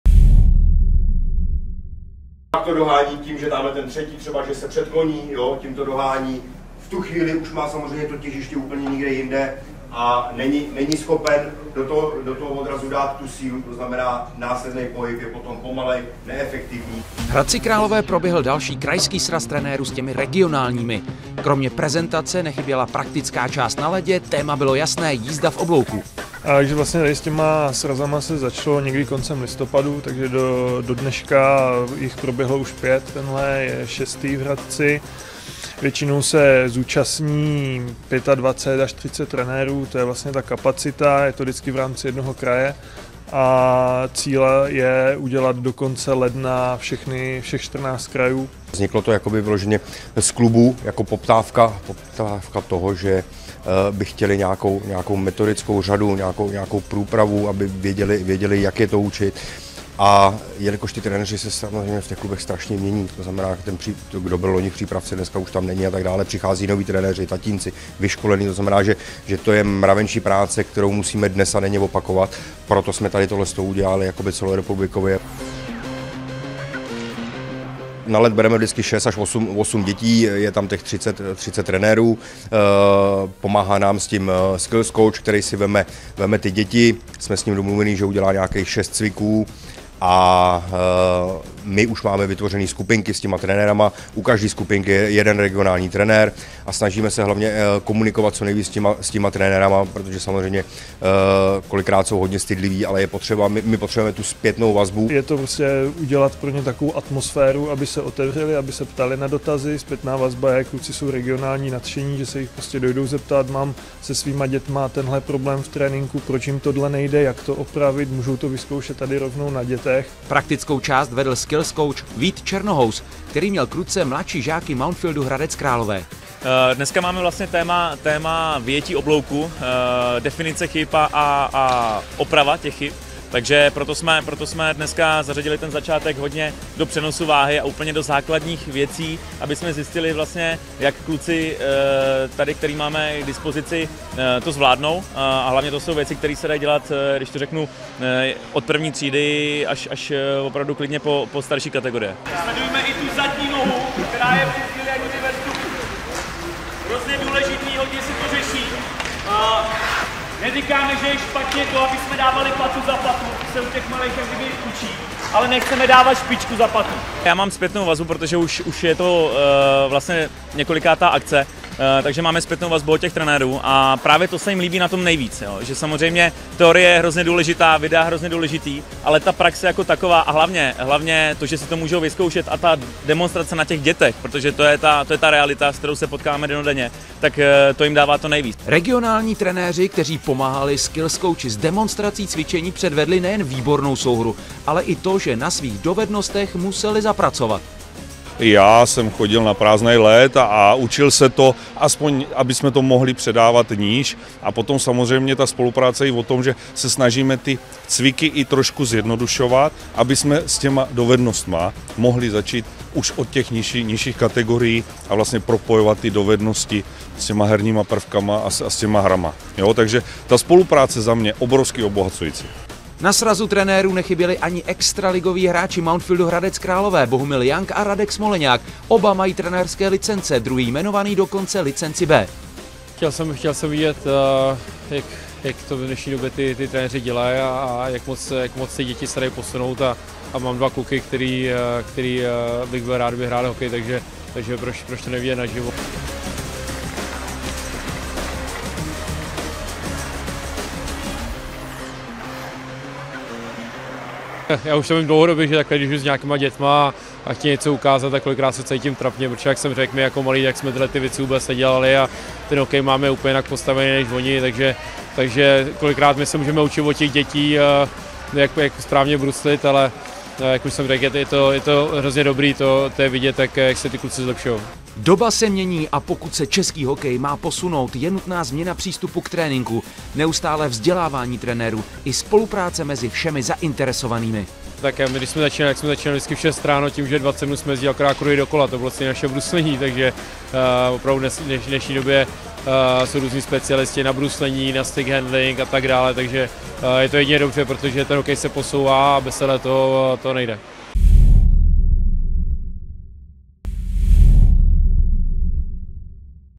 Nechyběli Smoleňák a Jank. Co se pilovalo na setkání s regionálními trenéry?
V letošní sezóně se pravidelně regionální trenéři scházejí s trenéry klubovými. Připravené téma se rozebírá nejen za pomoci videa, ale i přímo na ledě. Naposledy dostali teoretické informace, a také možnost vyzkoušet si všechno v praxi, trenéři z východních Čech. Teoreticky i prakticky se řešilo vyjetí oblouku. Mrkněte, jak probíhá spolupráce mezi trenéry, a proč se zapojili také extraligoví hráči Mountfieldu HK?